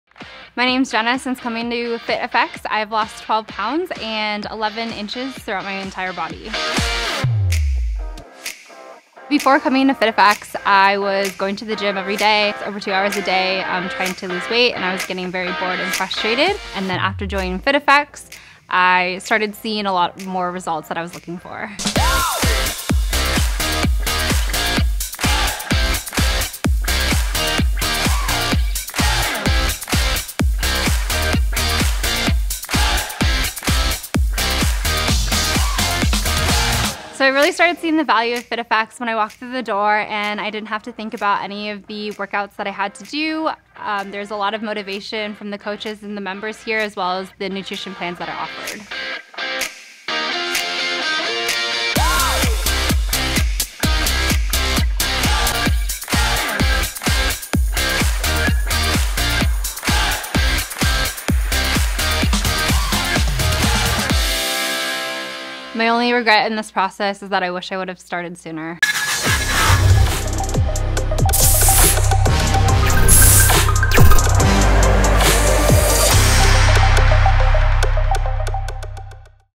FitFx member loses 12 pounds and 11 inches since joining.
My name is Jenna and since joining FitFX, I have lost 12 lbs and 11 inches throughout my entire body. Before joining FitFx, I used to go to the gym everyday for over 2 hours a day trying to lose weight but was getting really bored and frustrated with the results. Since joining FitFx, I have seen a lot more results that i was looking for. I really started seeing value of fitfx when i walked through the door and didn't have to think about any of the workouts that i had to do. There's a lot of motivation from coaches and members at this gym as well as nutrition plans offere. My only regret is that I wish I would have started sooner!